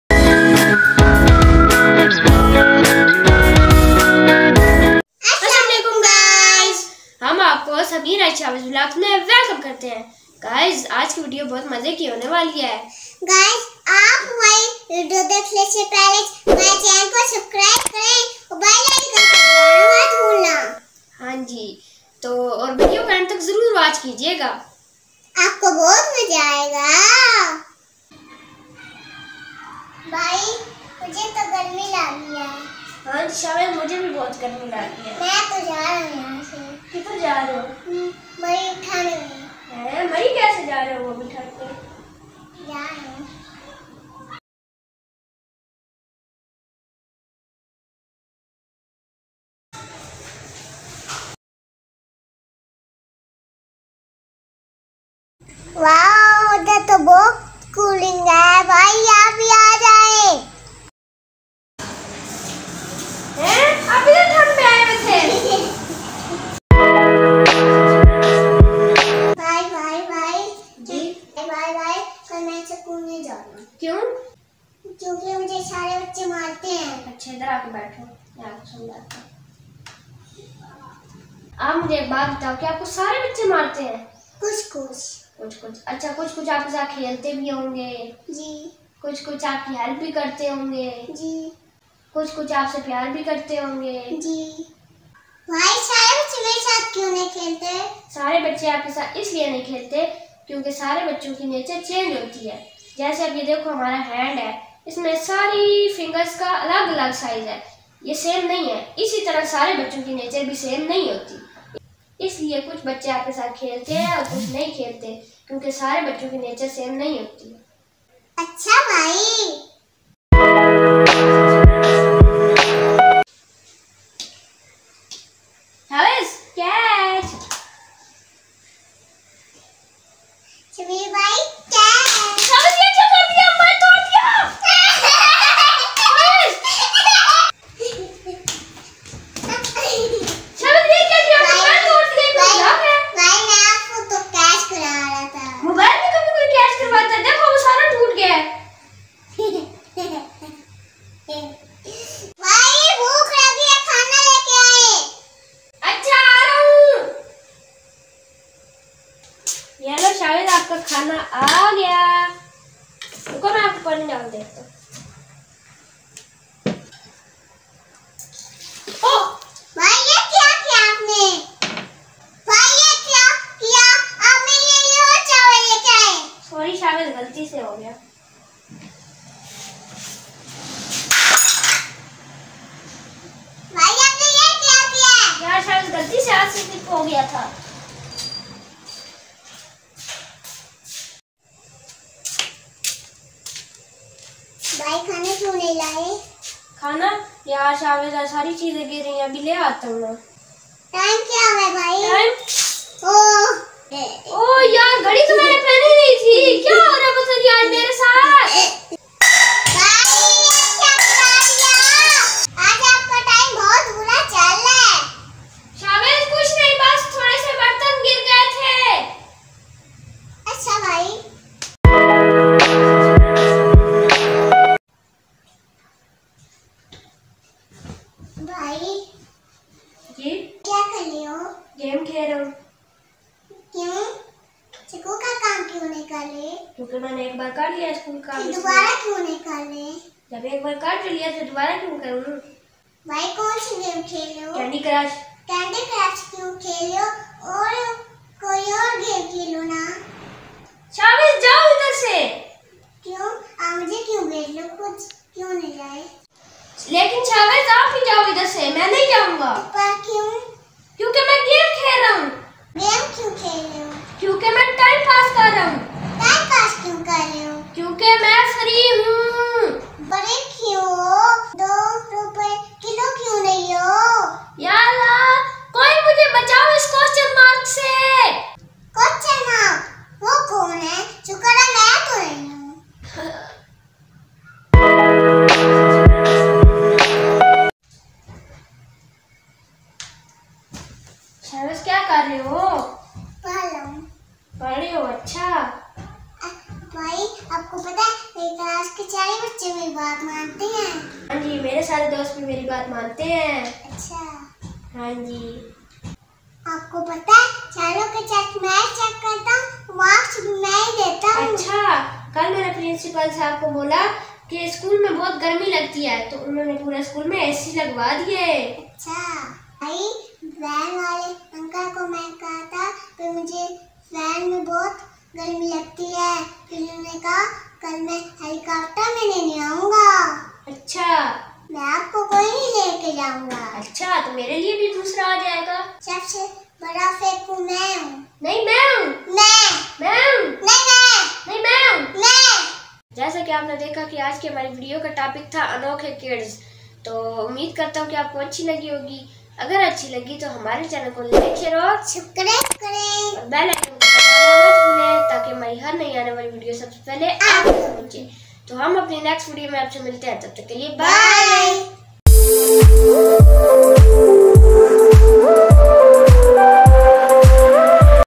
ANOKHE KIDS | Anokhay kids | Funny types of kids | Moral Story | Sameer & Shawaiz
aaj hum nay Anokhe kids ki video bnai is may mukhtalif kisam k bchon ki acting ki . bhot maza aya. video end tk watch kray bhot maza aya ga.@VillageLifeSameer
feku toru phoru#